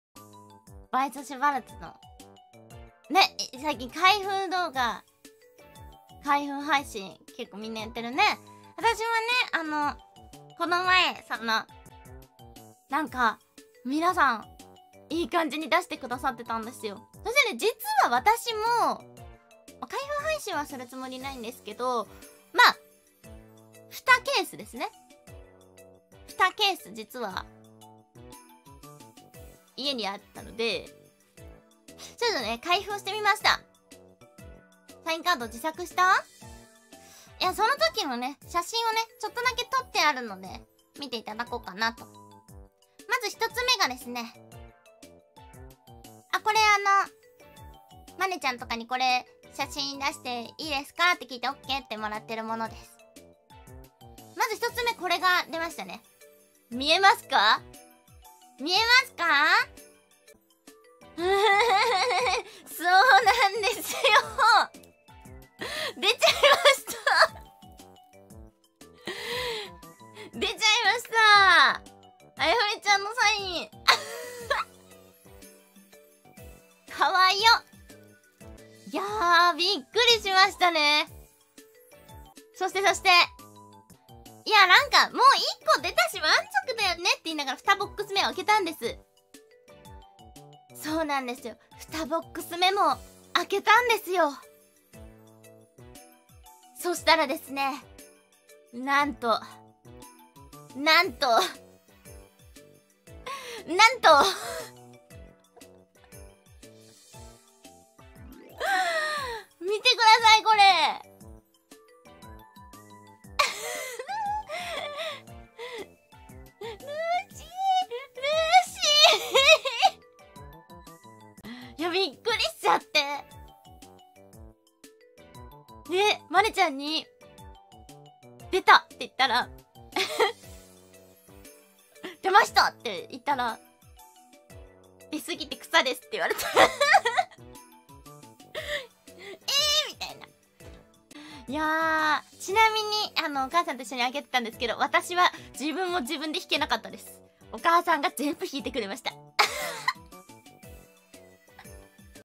【vtuber中翻精華】私底下開了WS卡包的空媽 居然抽到了...?【時乃空】
留言訂正
空媽是開了兩盒 而不是兩箱

原影片:
【雑談】いろんなお話をするのさ～【ときのそら生放送】